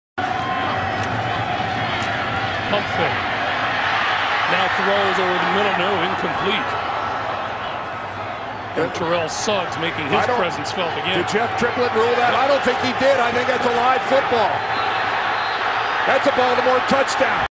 Cory Redding Touchdown 1/15/11 Ravens vs. Steelers Playoffs
Cory Redding picks up a fumble and returns it for a touchdown.
Ravens vs Steelers 2011